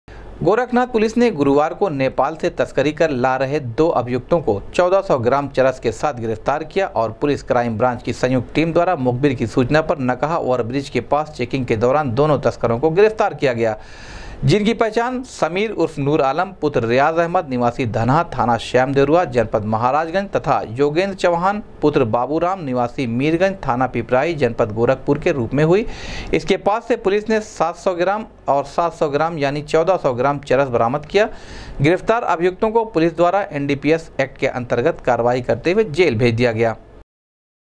14 सौ ग्राम चरस के साथ दो तस्कर चढ़े पुलिस और क्राइम ब्रांच के हत्थे, एनडीपीएस एक्ट
14 सौ ग्राम चरस के साथ दो तस्कर चढ़े पुलिस और क्राइम ब्रांच के हत्थे, एनडीपीएस एक्ट के तहत भेजे गए जेल